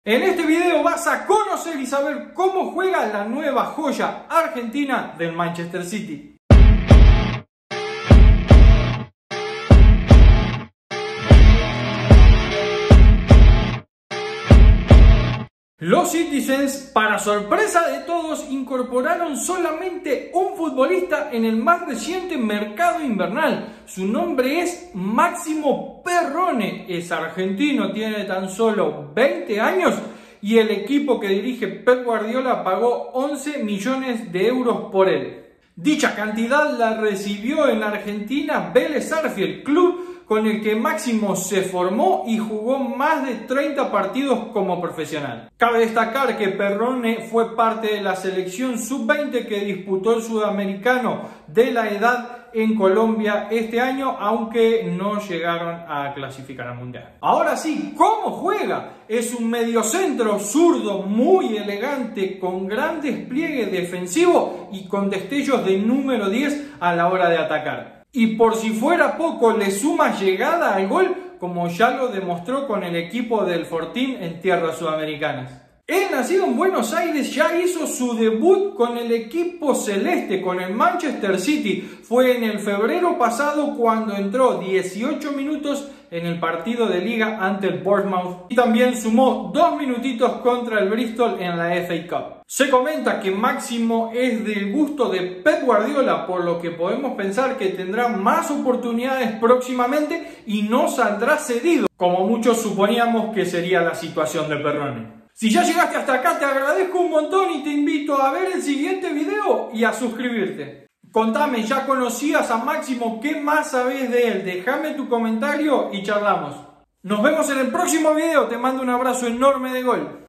ASÍ JUEGA LA NUEVA JOYA DEL MAN CITY 💎🇦🇷
Máximo Perrone fue adquirido por el club Citizen a cambio de 11 millones de euros, tiene 20 años y se perfila para ser una de las estrellas del equipo de Pep Guardiola.